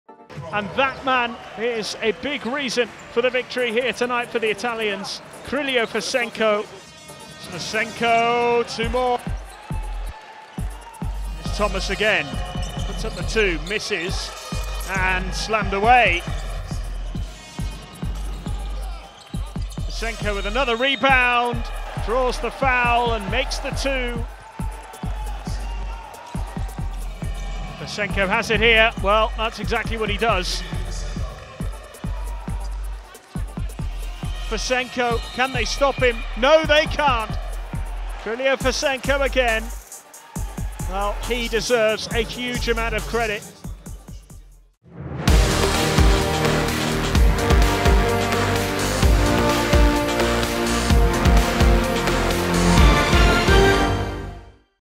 Kyrylo Fesenko is on fire! - High Double-Double and MVP of gameday 7!
Sidigas Avellino's Kyrylo Fesenko is unstoppable in the game against Juventus Utena. He scores sensational 25 points and made 13 rebounds with an efficiency rating of +35. All three were season highs for the Ukraine giant, who not surprisingly has scooped the honor as MVP of the Week.